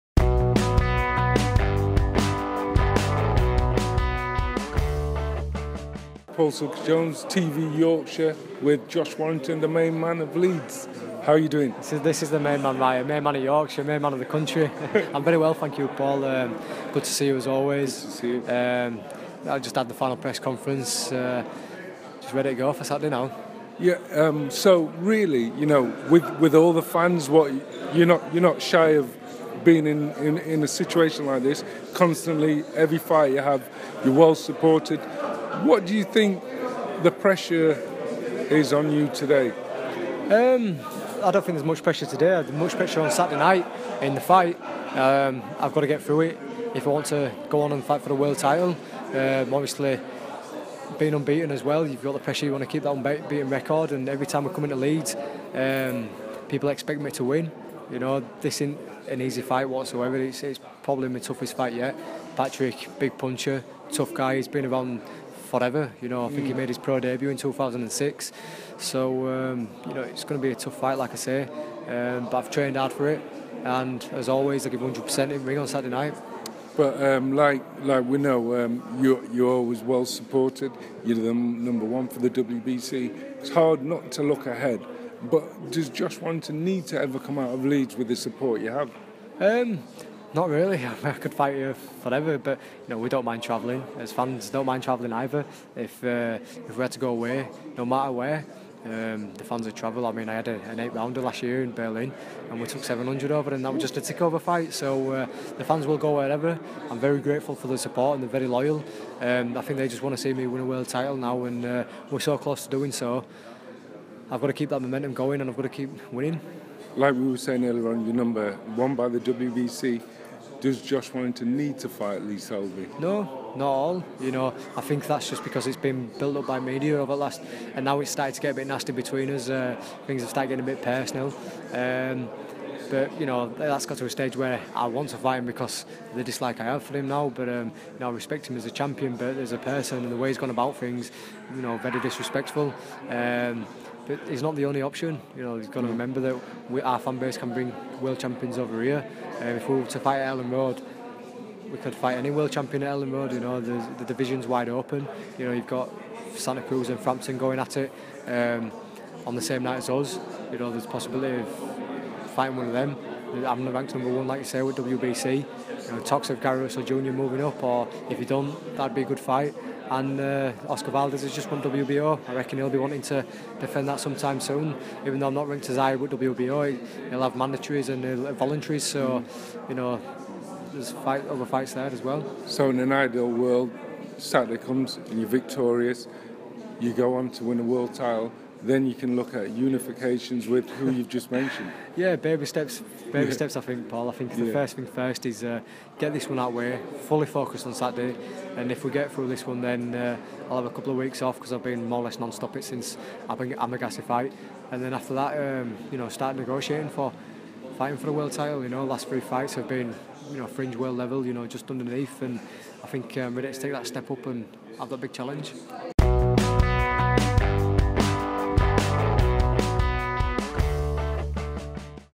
Warrington on Hyland & the possibility of the Selby fight
Paul Silky Jones speaks to the Leeds Warrior, Josh Warrington ahead of his fight against Patrick Hyland on Saturday night. PLUS he discusses the possibility of challenging for the world title against Lee Selby...